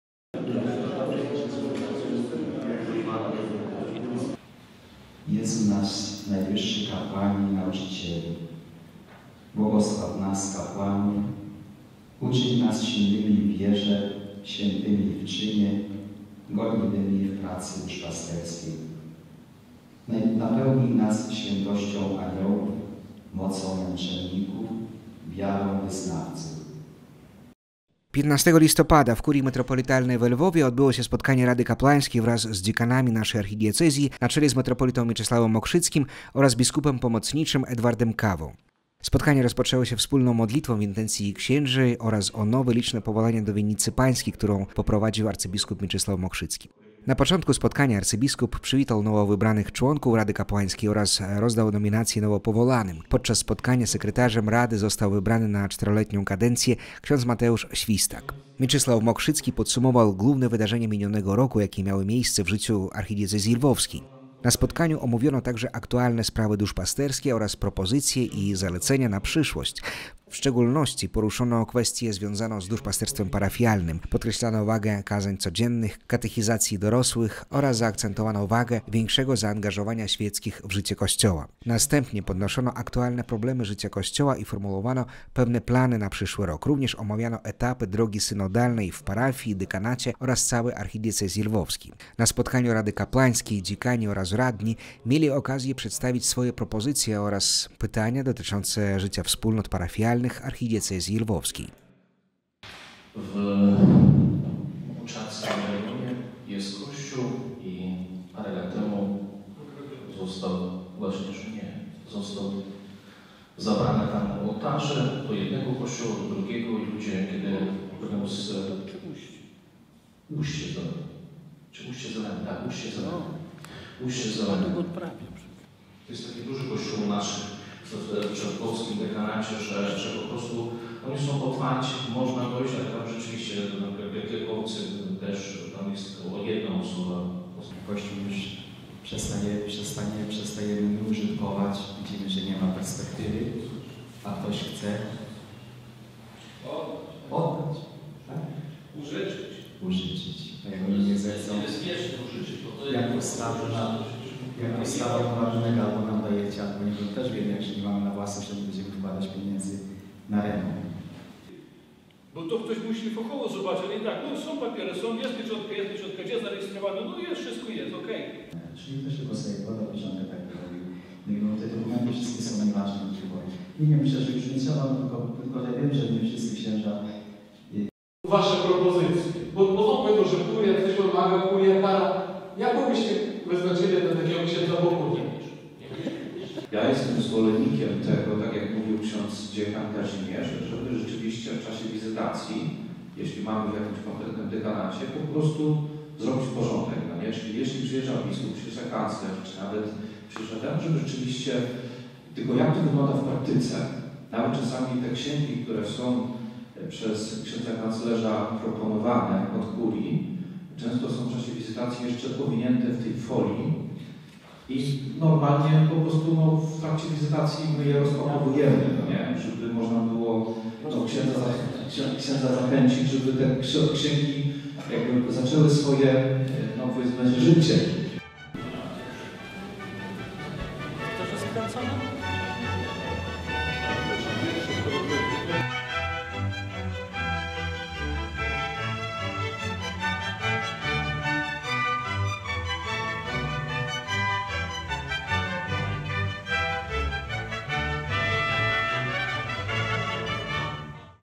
Spotkanie Rady Kapłanów i Dziekanów w Kurii Metropolitalnej
W dniu dzisiejszym, 15 listopada, o godz. 15.00 w Kurii Metropolitalnej odbyło się spotkanie Rady Kapłańskiej wraz z dziekanami naszej archidiecezji, na czele z metropolitą Mieczysławem i biskupem pomocniczym Edwardem Kawą. Na spotkanie przybyli wszyscy członkowie Rady księża i dziekani.

Spotkanie rozpoczęło się wspólną modlitwą w intencji księży i o nowe, liczne powołania do winnicy Pańskiej, którą odmówił abp Mieczysław. Na początku spotkania arcybiskup Mokrzycki przywitał nowo wybranych członków Rady Kapłańskiej i rozdał nominacje nowo powołanym. Podczas spotkania sekretarzem Rady został wybrany na czteroletnią kadencję ks. Mateusz Świstak.

Arcybiskup podsumował główne wydarzenia minionego roku, jakie miały miejsce w życiu archidiecezji. Na spotkaniu omówiono aktualne sprawy duszpasterskie oraz propozycje i zalecenia na przyszłość. W szczególności poruszali kwestie związane z duszpasterstwem parafialnym: kazania codzienne, katechezę dorosłych, zaangażowanie świeckich w życie Kościoła.
Następnie podnoszono aktualne problemy życia Kościoła i formułowano pewne plany na przyszły rok, również omawiano etapy drogi synodalnej w parafii, dekanacie i całej diecezji. Dziekani i radni mieli okazję przedstawić swoje propozycje i pytania dotyczące życia wspólnot parafialnych.